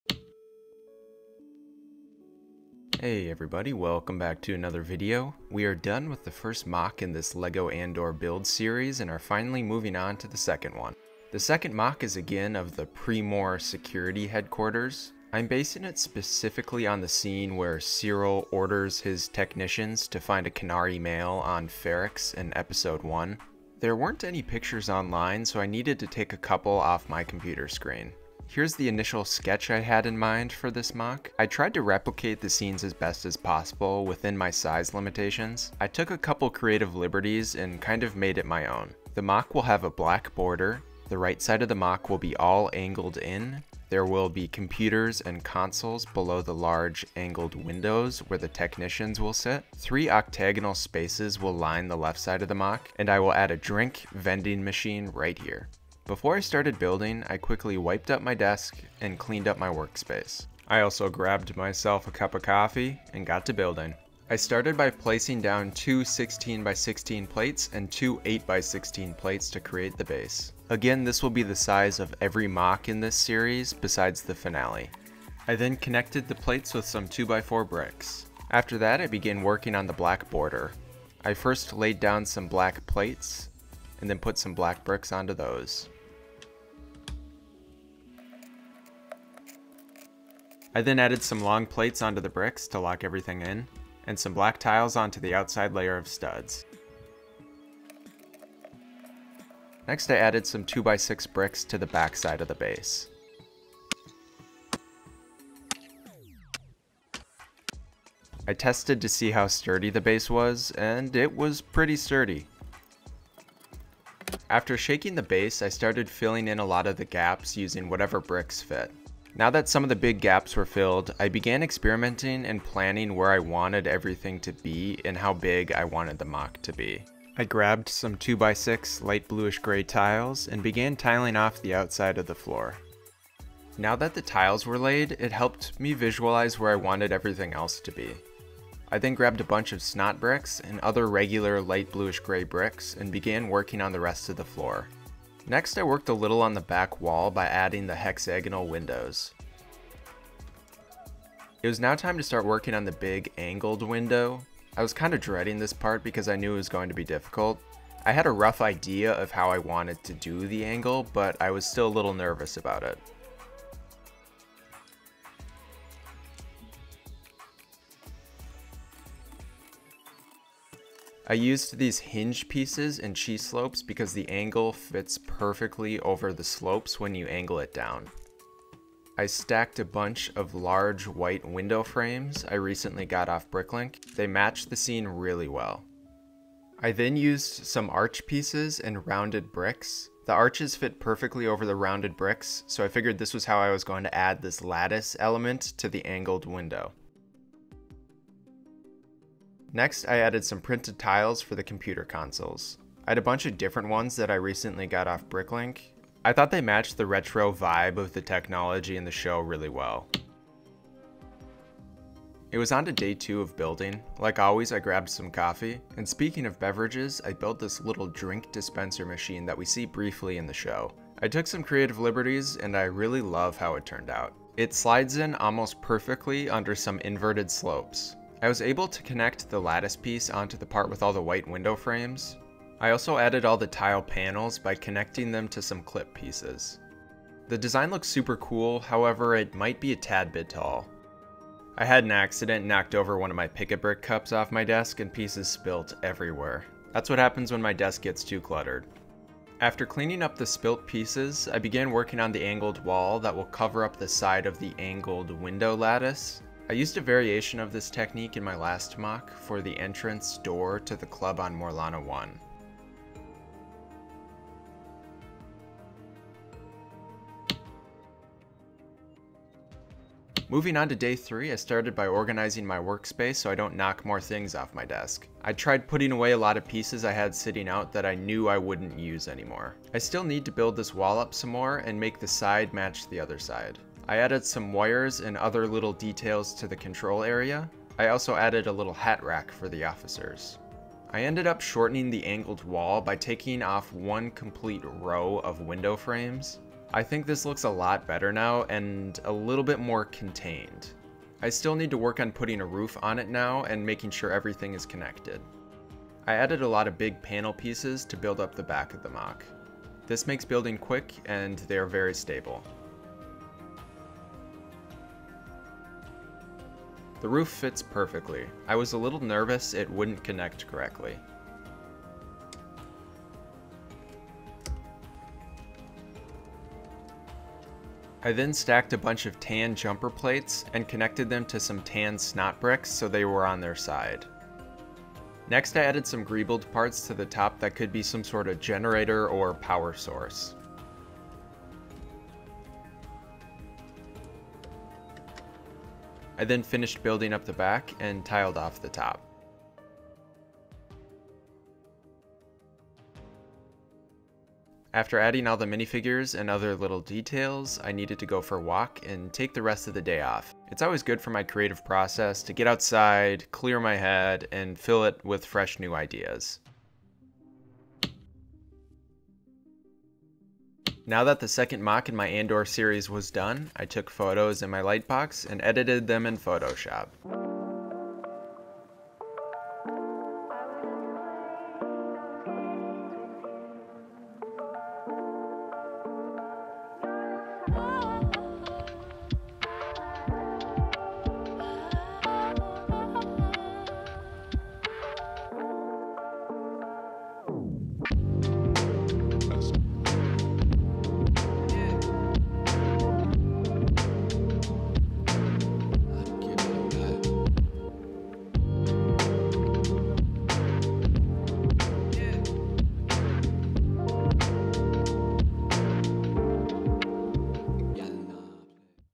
I Build a LEGO STAR WARS Andor MOC - Pre-Mor Security HQ
Hello Everyone!

I am back with another LEGO Star Wars build video. Today I am continuing my Andor build series where I will be creating 12 LEGO Star Wars MOCS depicting my favorite scenes from the TV show Andor. In this video I will be building my second Andor MOC. This MOC is of the Pre-Mor Security Headquarters on Morlana One. It took me about three days to build. I hope you learn something!
Support me?